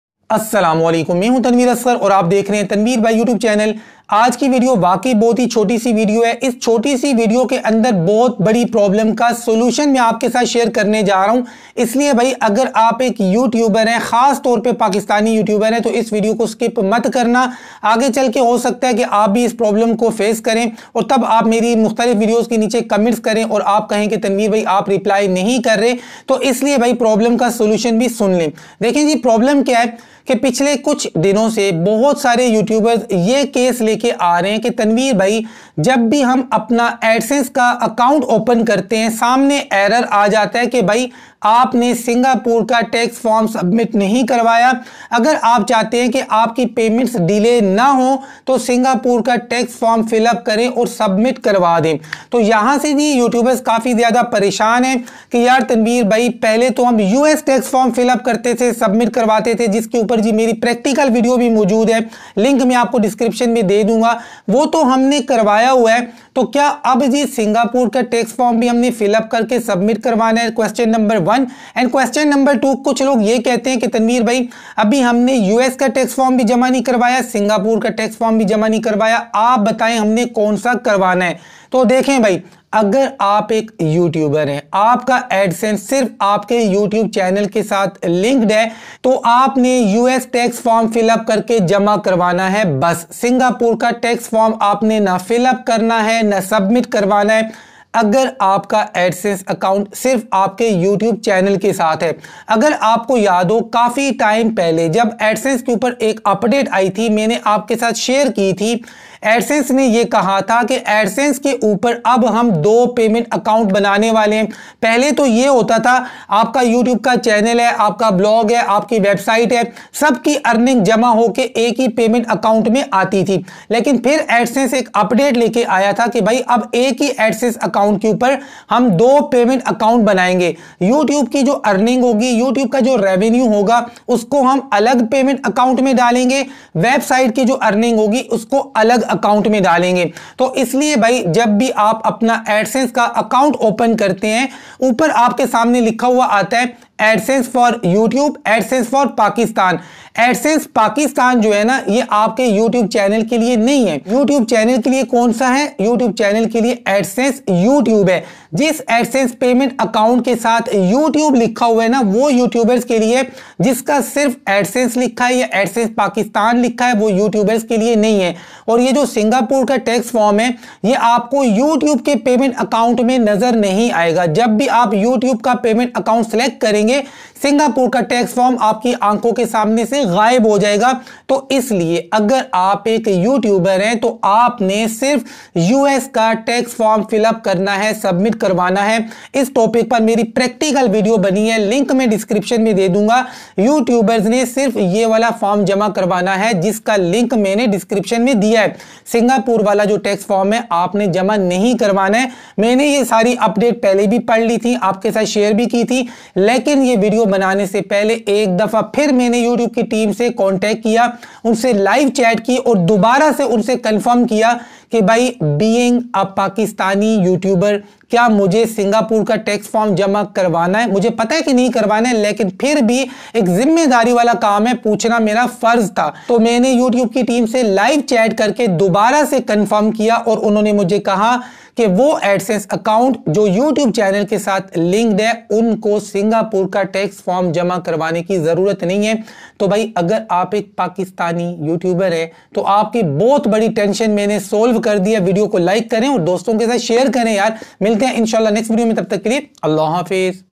Singapore Tax info on AdSense | AdSense For YouTube
How to Submit Singapore Tax info in my AdSense account. there are two payment accounts on AdSense 1st one is for "AdSense' and the 2nd one is "AdSense for YouTube" if you are a YouTuber then you need to Submit the US Tax Info in AdSense Account. Singapore Tax Form is not for YouTubers.

Singapore Tax Info